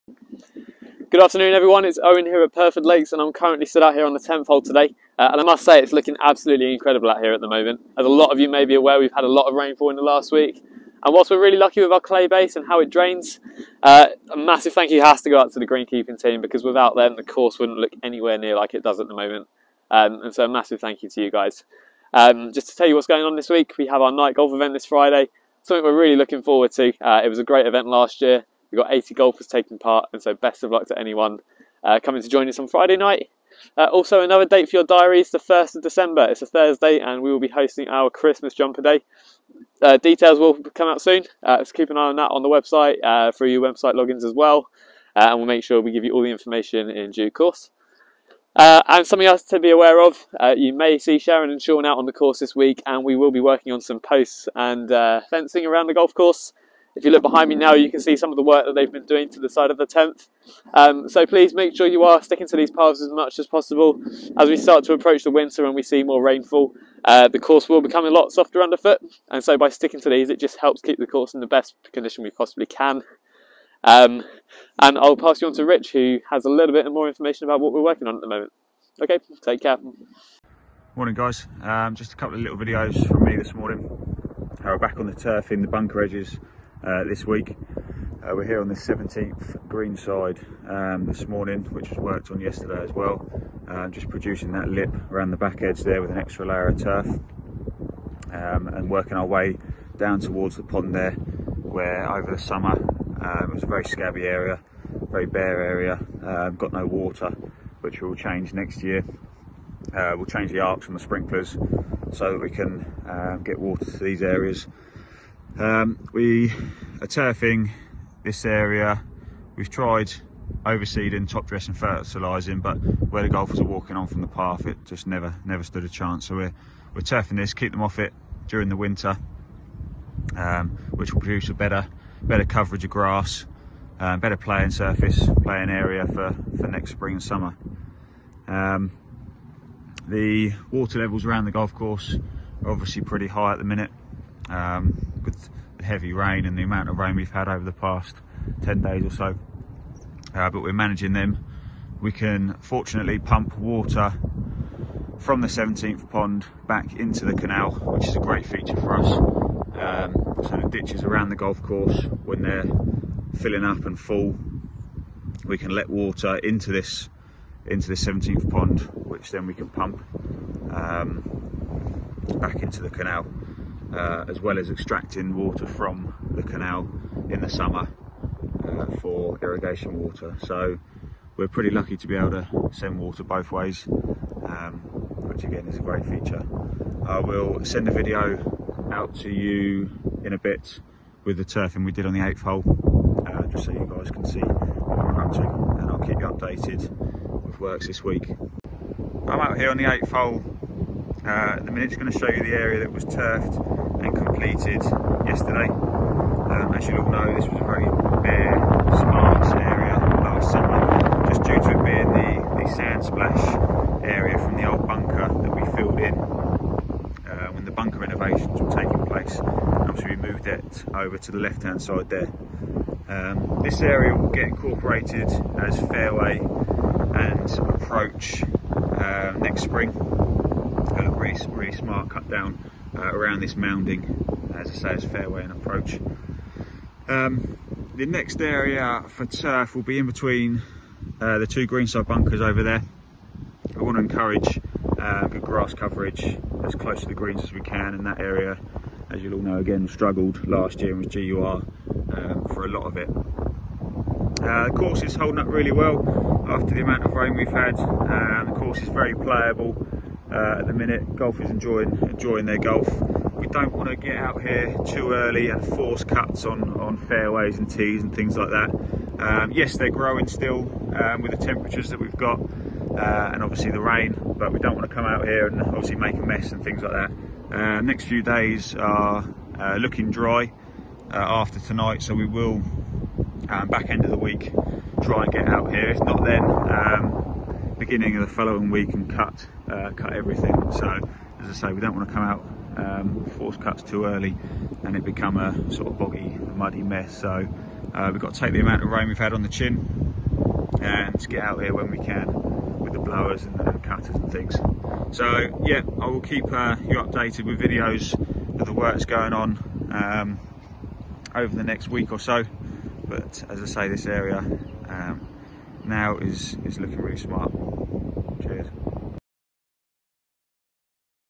Members Update - Week 2 (November)
Owen and Rich take you through what’s been happening recently at Pyrford Lakes